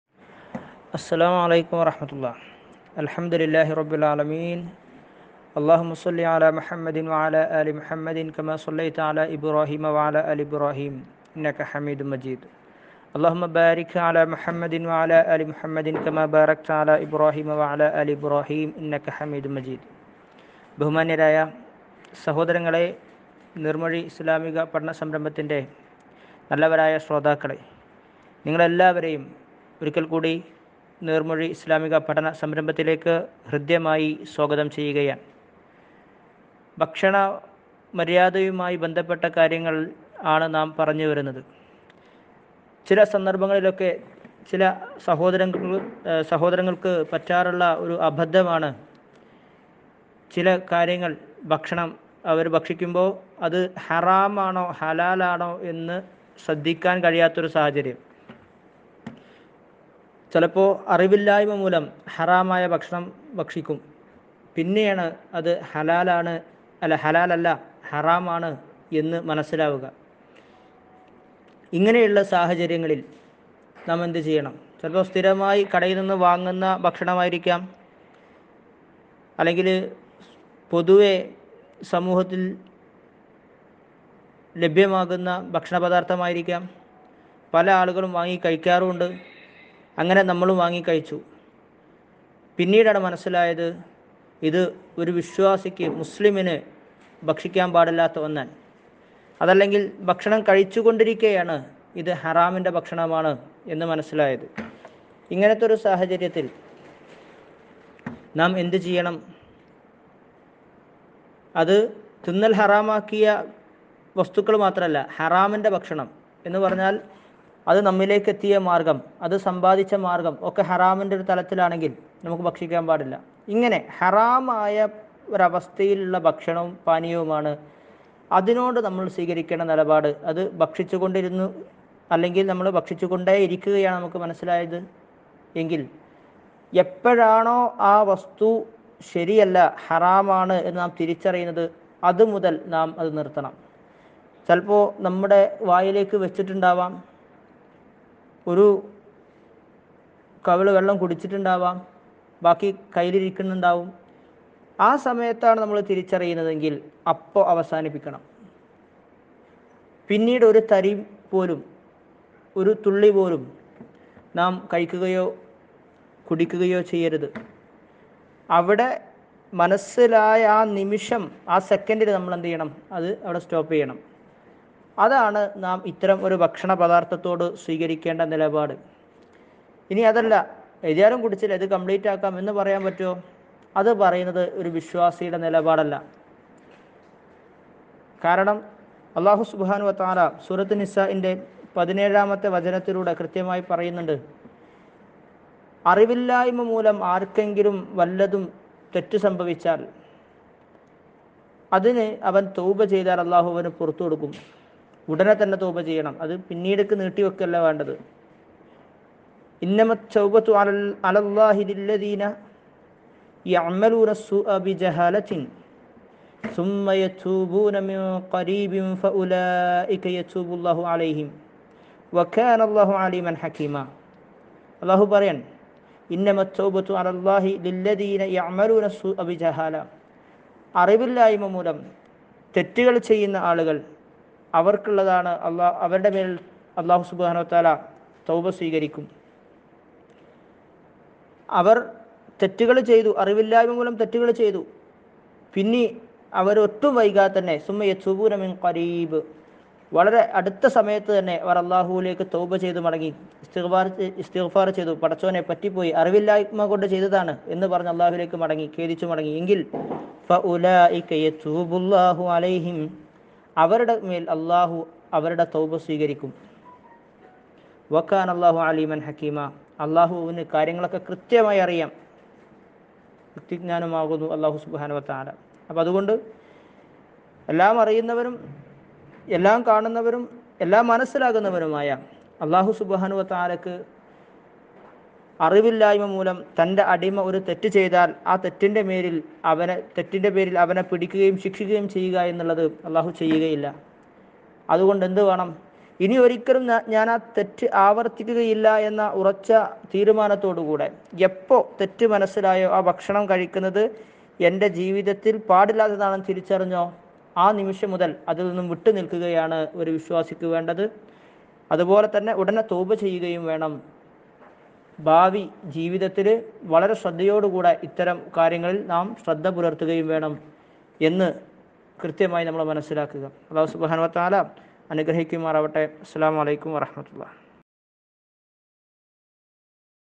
ഹറാമായതാണെന്ന് തിരിച്ചറിഞ്ഞാൽ!! Iഭക്ഷണ മര്യാദകൾ 11 INermozhi
⏳ചില സന്ദർഭങ്ങളിലൊക്കെ ചിലർക്ക് പറ്റുന്ന അബദ്ധമാണ് അവർ ഭക്ഷിക്കുന്ന ഭക്ഷണം ഹറാമാണോ ഹലാലാണോ എന്ന് ശ്രദ്ധിക്കാൻ കഴിയാത്ത സാഹചര്യം.. അറിവില്ലായ്മ മൂലമാകാം.. ചില അവസരത്തിൽ ഹറാമായ ത് ഭക്ഷിക്കും. പിന്നീട് അത് ഹറാമായിരുനെന്ന് മനസ്സിലാകുന്നതും. ഇങ്ങനെയുള്ള സാഹചര്യങ്ങളിൽ നാം എന്ത് ചെയ്യണം?
‼️ചിലപ്പോൾ നാം സ്ഥിരമായും വാങ്ങുന്ന ഭക്ഷണമായിരിക്കാം. പിന്നീടാണ് അറിഞ്ഞത് ഇതൊരു വിശ്വാസിക്ക് ഹറാമാണെന്ന്.. അതുമല്ലെങ്കിൽ കഴിച്ചു കൊണ്ടിരിക്കെ അറിഞ്ഞാലും ഇങ്ങനെയുള്ള സാഹചര്യത്തിൽ നാം എന്തുചെയ്യണം. അത് ഹറാമാക്കിയ വസ്തുക്കൾ മാത്രമല്ല, അത് നമ്മിലേക്ക് എത്തിയ മാർഗ്ഗവവും ഇതിൽ പെടുന്നു. ഹറാം സമ്പാദിച്ചതിൽ ആയാലും ഇതിലുൾപ്പെടും.
❓എപ്പോഴാണ് ആ വസ്തു ഹറാമാണെന്ന് നാം തിരിച്ചറിഞ്ഞത് ആ നിമിഷം മുതൽ അത് നാം അവസാനിപ്പിക്കണം. പിന്നീട് അതിന്റെ ഒരു തരിപോലും നമ്മുടെ അകത്തേക്ക് ചെല്ലരുത്. അത് അറിഞ്ഞിട്ടും  പിന്നെയും അത് പൂർത്തിയാക്കുകയെന്നുള്ളത് വിശ്വാസിക്ക് ചേർന്നതല്ല.
അല്ലാഹു പറയുന്നു
إِنَّمَا ٱلتَّوْبَةُ عَلَى ٱللَّهِ لِلَّذِينَ يَعْمَلُونَ ٱلسُّوٓءَ بِجَهَٰلَةٍ ثُمَّ يَتُوبُونَ مِن قَرِيبٍ فَأُو۟لَٰٓئِكَ يَتُوبُ ٱللَّهُ عَلَيْهِمْ ۗ وَكَانَ ٱللَّهُ عَلِيمًا حَكِيمًا ﴾١٧﴿
▪️നിശ്ചയമായും, അല്ലാഹുവിന്‍റെമേല്‍ പശ്ചാത്താപം സ്വീകരിക്കല്‍ (ബാധ്യത) ഉള്ളത്, വിവിഡ്ഢിത്തം നിമിത്തം തിന്മ പ്രവര്‍ത്തിക്കുകയും, പിന്നീട് അടുത്തു (തന്നെ) പശ്ചാത്തപിക്കുകയും ചെയ്യുന്നവര്‍ക്ക് (മാത്രം) ആകുന്നു. എന്നാല്‍, അക്കൂട്ടര്‍- അവരുടെ മേല്‍ അല്ലാഹു പശ്ചാത്താപം സ്വീകരിക്കുന്നതാണ്. അല്ലാഹു സര്‍വ്വജ്ഞനും അഗാധജ്ഞനുമാകുന്നു
സൂ -4:17📚
📌അവിവേകവും, വിഡ്ഢിത്തവും കാരണം തിന്മകള്‍ ചെയ്‌തേക്കുക മനുഷ്യസഹജമായ സ്വഭാവമാകുന്നു. എന്നാല്‍, കുറ്റകരമായ കാര്യങ്ങള്‍ പ്രവര്‍ത്തിച്ചശേഷം താമസിയാതെ അതില്‍നിന്ന് ഖേദിച്ച് മടങ്ങുന്നവരുടെ പശ്ചാത്താപം അല്ലാഹു സ്വീകരിക്കാതിരിക്കുകയില്ല. നേരെ മറിച്ച് കുറ്റങ്ങള്‍ ചെയ്തുകൊണ്ടിരിക്കുകയും മരണം വരുമ്പോള്‍ ഖേദം പ്രകടിപ്പിച്ച് മടങ്ങുകയും ചെയ്യുന്നവരുടെയും, അവിശ്വാസിയായിക്കൊണ്ട് മരണപ്പെടുന്നവരുടെയും പശ്ചാത്താപം അല്ലാഹുവിന്‍റെ അടുക്കല്‍ സ്വീകാര്യമായിരിക്കുകയുമില്ല എന്നത്രെ ഈ വചനങ്ങളില്‍ പ്രസ്താവിച്ചതിന്‍റെ ചുരുക്കം. പശ്ചാത്താപം എന്ന് അര്‍ഥം കല്‍പിച്ച تَوْبَة എന്ന വാക്കിന്‍റെ സാക്ഷാല്‍ അര്‍ഥം ‘മടക്കം, ഖേദം’ എന്നൊക്കെയാണെന്നും, അല്ലാഹുവിനെ സംബന്ധിച്ചിടത്തോളം മടക്കം കൊണ്ടുദ്ദേശ്യം പശ്ചാത്താപം സ്വീകരിക്കുക എന്നാണെന്നും മുമ്പ് നാം പ്രസ്താവിച്ചിട്ടുണ്ടല്ലോ.

💡വിഡ്ഢിത്തം നിമിത്തം തെറ്റ് ചെയ്യുകയും, ഉടനെ പശ്ചാത്തപിക്കുകയും ചെയ്യുന്നവരുടെ പശ്ചാത്താപം മാത്രമേ സ്വീകരിക്കപ്പെടുകയുള്ളൂവെന്നല്ല  അങ്ങിനെയുള്ളവരുടെ പശ്ചാത്താപം സ്വീകരിക്കല്‍ മാത്രമേ അല്ലാഹുവിന് ബാധ്യതയുള്ളൂ എന്നത്രെ അല്ലാഹു പറഞ്ഞത്.

📜അപ്പോള്‍, അങ്ങിനെയല്ലാത്തവരുടെ കല്പിച്ചുകൂട്ടി തെറ്റു ചെയ്യുകയും, ഉടനെത്തന്നെ പശ്ചാത്തപിക്കാതിരിക്കുകയും ചെയ്യുന്നവരുടെ പശ്ചാത്താപം അല്ലാഹു സ്വീകരിച്ചെന്നും, സ്വീകരിച്ചില്ലെന്നും വരാം. രണ്ടും അവന്‍റെ ഉദ്ദേശ്യം അനുസരിച്ചായിരിക്കും സംഭവിക്കുക എന്നത്രെ താല്‍പര്യം. തന്നില്‍ പങ്കുചേര്‍ക്കുക (شرك) എന്നതല്ലാത്ത എല്ലാ പാപങ്ങളും പൊറുക്കപ്പെടാവുന്നതാണെന്നും, അവയില്‍നിന്നുള്ള പശ്ചാത്താപം അല്ലാഹു ഉദ്ദേശിക്കുന്നവരില്‍നിന്ന് അവന്‍ സ്വീകരിക്കുന്നതാണെന്നും അല്ലാഹു വാഗ്ദാനം ചെയ്തിട്ടുള്ളതാണല്ലോ. (48, 116 എന്നീ വചനങ്ങളും, 3:135. മുതലായ വചനങ്ങളും നോക്കുക). നബി (സ.അ) അരുളിച്ചെയ്തതായി ഇബ്‌നുഉമര്‍ (റ) ഇപ്രകാരം ഉദ്ധരിക്കുകയും ചെയ്തിരിക്കുന്നു: ‘നിശ്ചയമായും അടിയാനു (മനുഷ്യന്) മരണം തൊണ്ടക്കുഴിയിലെത്താത്തപ്പോഴെല്ലാം അവന്‍റെ പശ്ചാത്താപം അല്ലാഹു സ്വീകരിക്കുന്നതാണ്.’ (അ; തി; ജ; ഹാ) പക്ഷേ, കഴിവതും വേഗത്തില്‍തന്നെയുള്ള പശ്ചാത്താപമായിരിക്കും കൂടുതല്‍ സ്വീകാര്യമായിത്തീരുക എന്നു പറയേണ്ടതില്ല. മരണവേളയിലെ ഖേദംകൊണ്ട് പ്രയോജനമില്ലെന്നും, അവിശ്വാസികളായി മരണപ്പെടുന്നവരുടെ പാപങ്ങള്‍ പൊറുക്കുകയില്ലെന്നും അല്ലാഹു വ്യക്തമായി പ്രസ്താവിച്ചിട്ടുള്ളതുമാകുന്നു.ഭാവി ജീവിതത്തിൽ ഇത്തരം കാര്യങ്ങളിൽ ശ്രദ്ധപുലർത്തുകയും വേണം.
▪️റബ്ബ് അനുഗ്രഹിക്കട്ടെ ആമീൻ